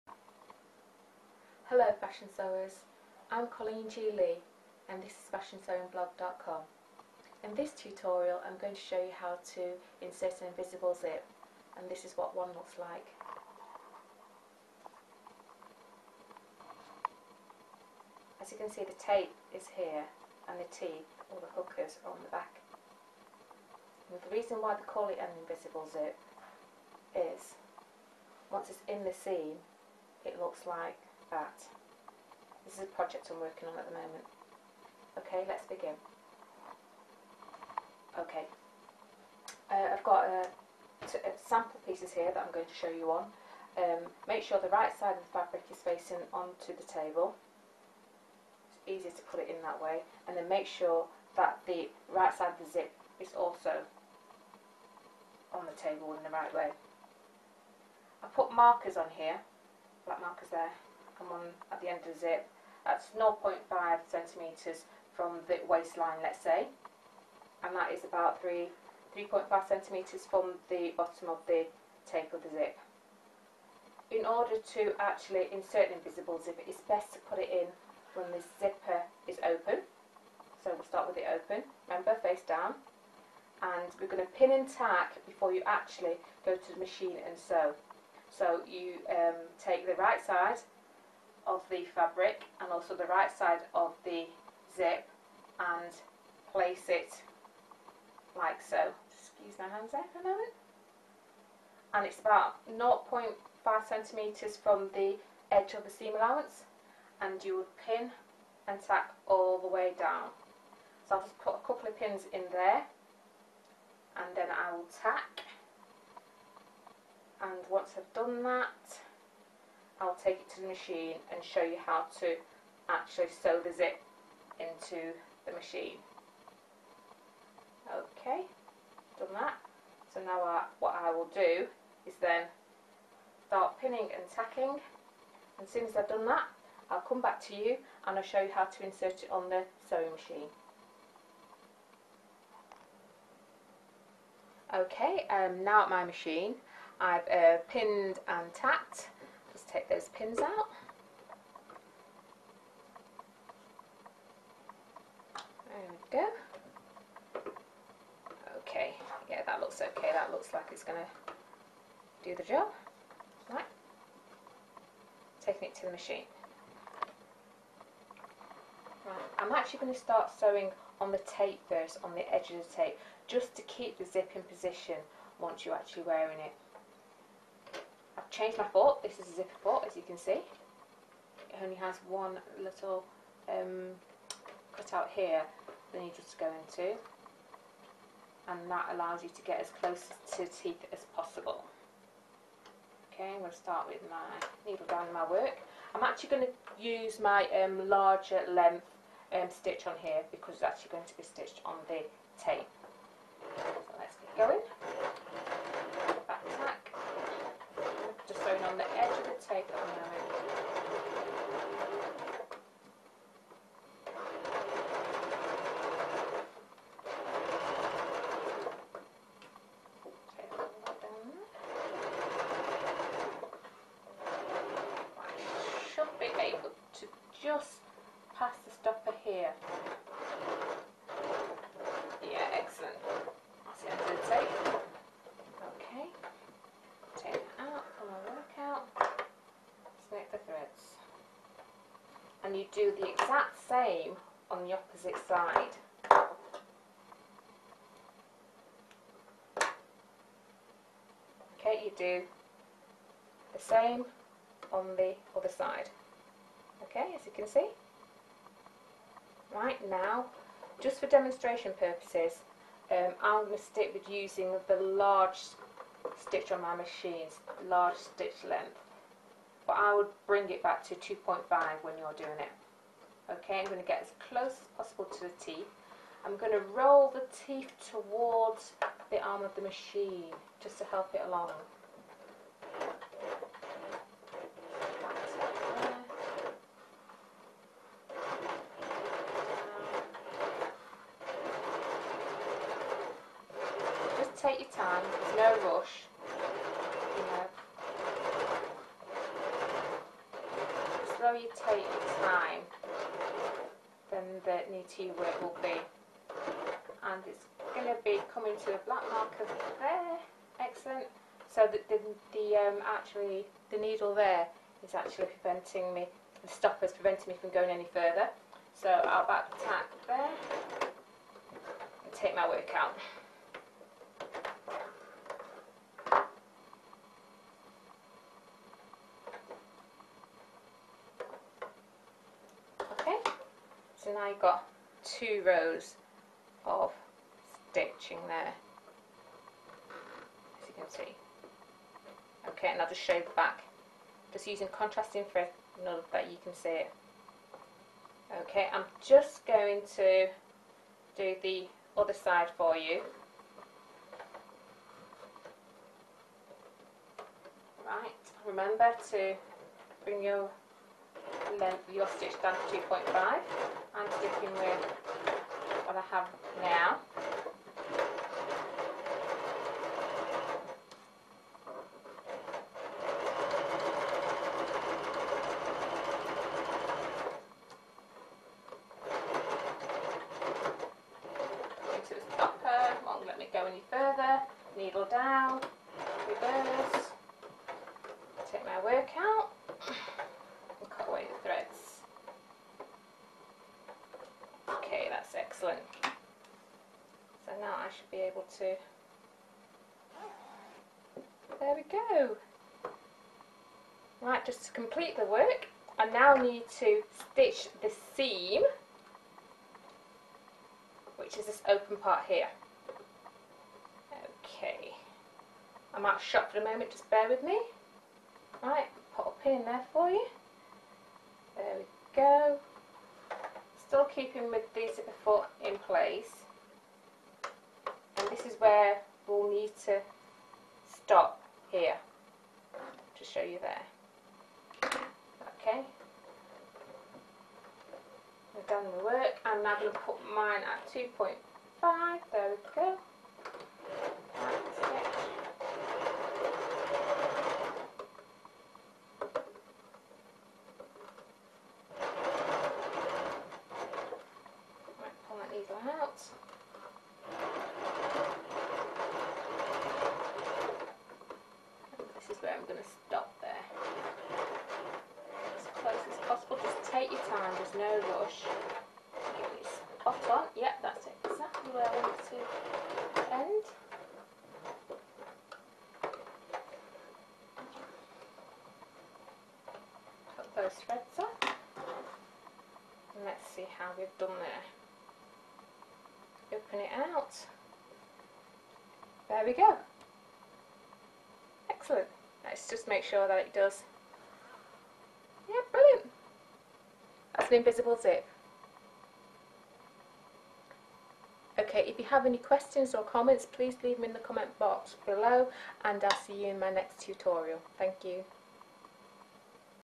Sewing an Invisible Zipper | Using a Zipper Foot
Sewing an Invisible Zipper | Using a Zipper Foot. An easy to follow tutorial on how to insert an invisible zipper into an open seam.  The tip here is to remember that the rest of the seam will be stitched after the zipper is in place.  These step by step instructions show just how easy it is to do without the use of a zipper foot on a sewing machine.  This technique will give the very best results and it is an easy method for advanced beginners to challenge themselves even when completing more advanced sewing projects

SUPPLIES USED IN THIS VIDEO TUTORIAL

* Invisible zippers